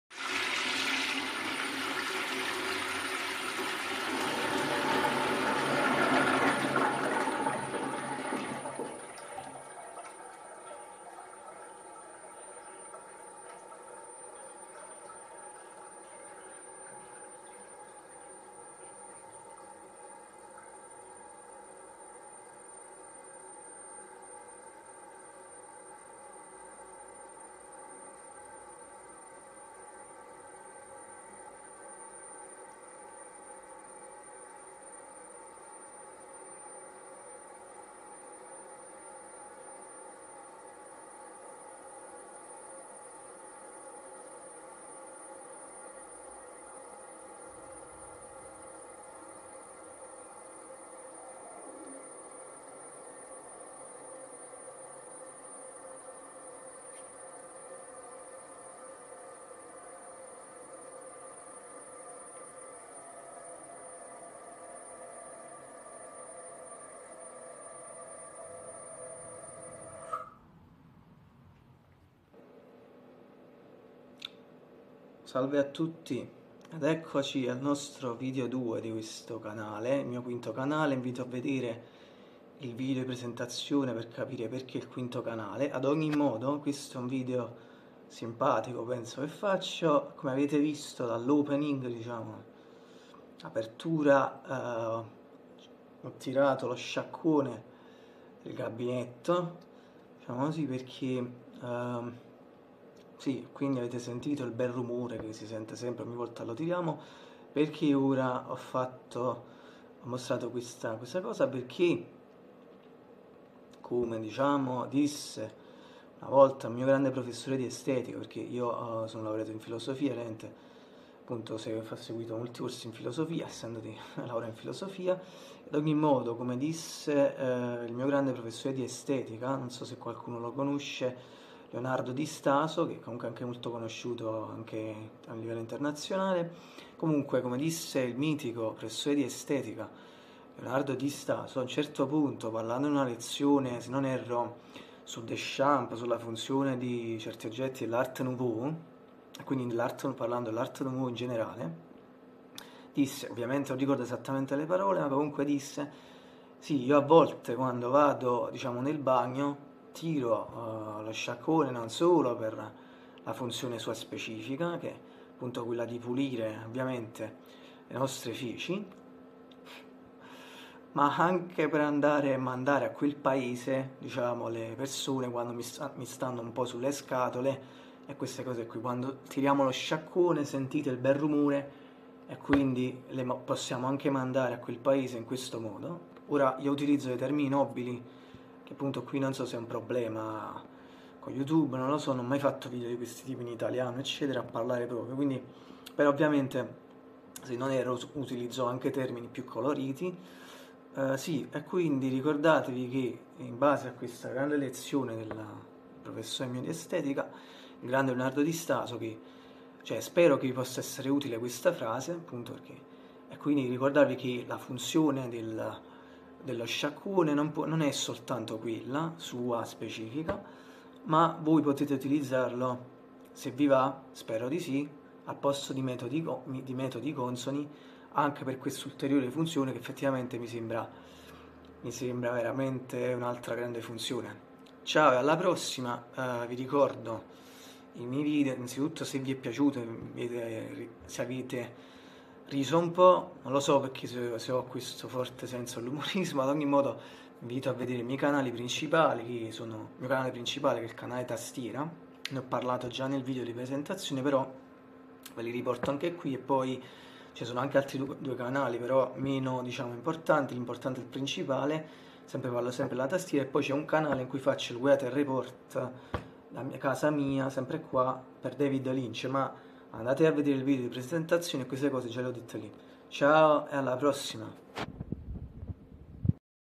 Video 2: E come disse il mio grande professore d'Estetica "Lo sciacquone serve anche per.."
00:00 Classica tirata dello sciacquone, che apparentemente serve solo a questo;
01:14 Sì, ma come disse il mio professore d'Estetica in realtà può servire anche per..